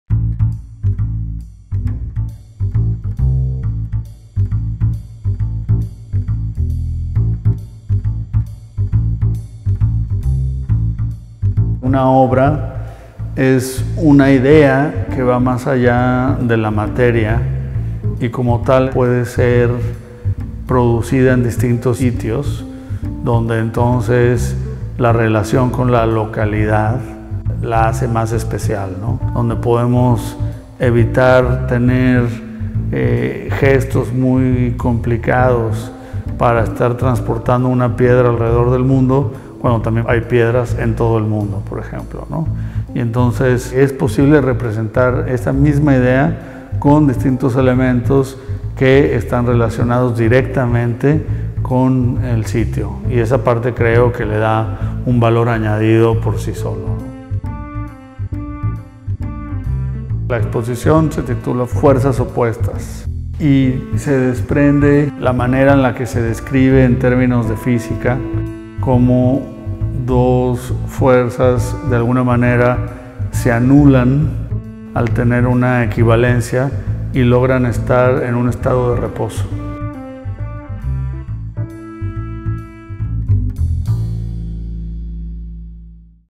Jose Dávila, Fuerzas Opuestas
Kewenig is delighted to present the first exhibition of Jose Dávila with the gallery in our Palma space. Entitled ‘Fuerzas Opuestas’, Dávila’s monumental installation was conceived especially for the oratory, building a dialogue between his raw materials and the unique architecture of the 13th-century chapel.

Videographer: Elena Rotger
Fix photography: Guillem Alzina